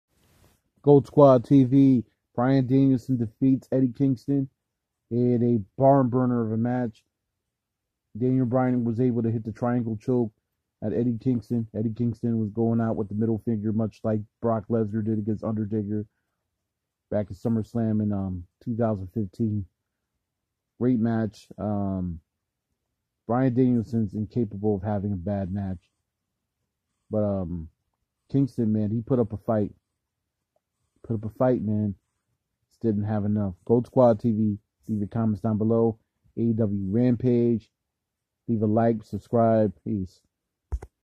Brian Danielson defs Eddie Kingston
Brian Daneilson defs Eddie Kingston is a crazy macth by submission

Daneilson advances in Aew World title tournament